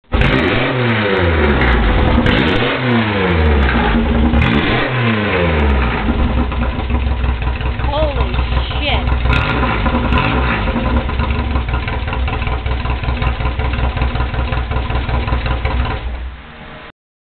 1st engine run
i ran my bug at around 2500 to 3500 for 15 minutes and this is revving it after that
its a 2110 with mini wedge port heads, dual 40 webers an engle 120 cam